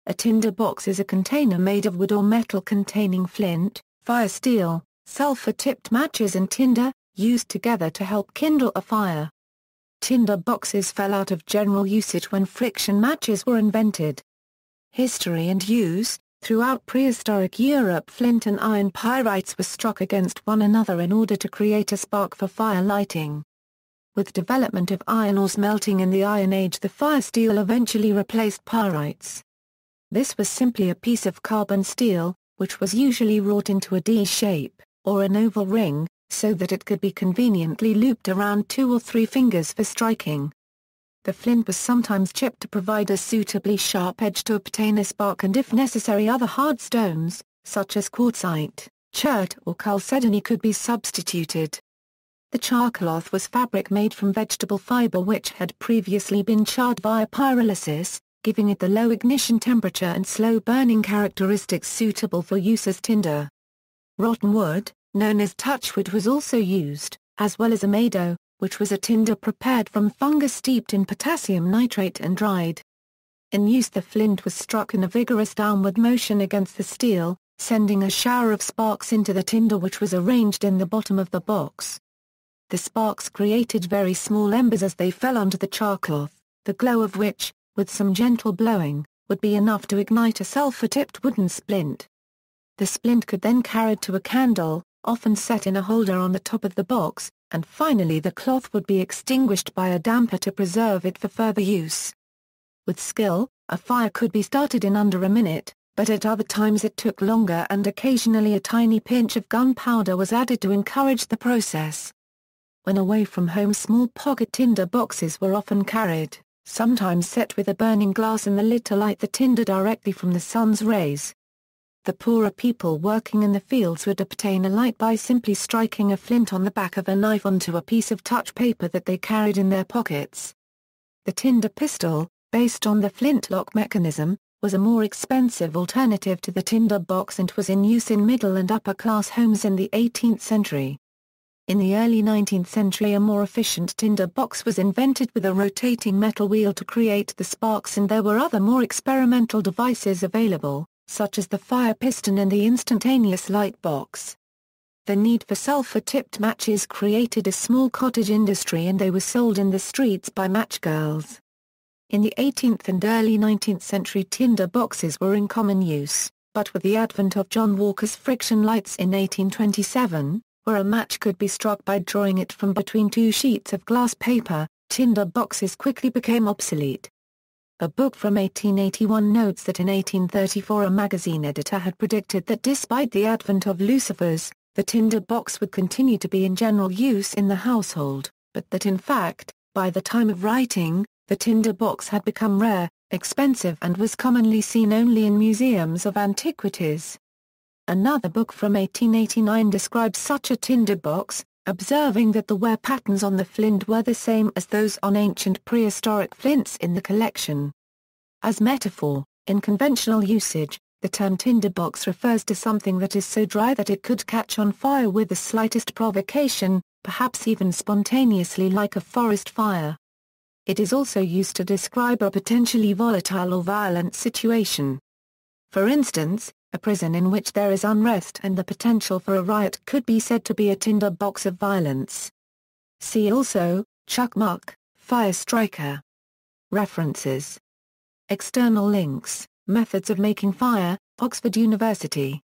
Tinderbox
A tinderbox is a container made of wood or metal containing flint, firesteel, sulphur tipped matches and tinder, used together to help kindle a fire.
Tinderboxes fell out of general usage when friction matches were invented.